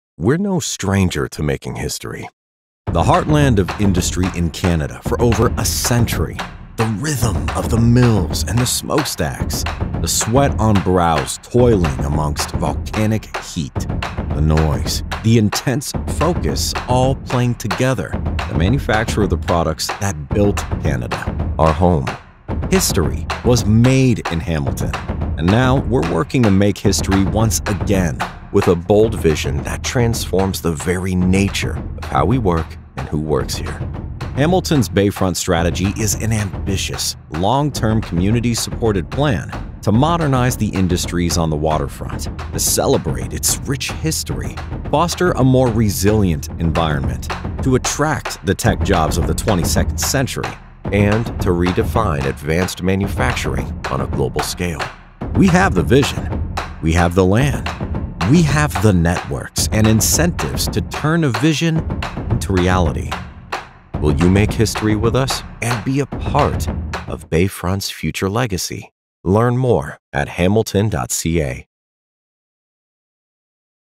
The Bayfront Industrial Area Strategy
The Bayfront Industrial Area Strategy is a 45+ year vision and action plan that seeks to encourage efficient use of land, attract growth and investment, and improve the environmental conditions and image of the City’s largest and oldest industrial area for a future of continuing productivity.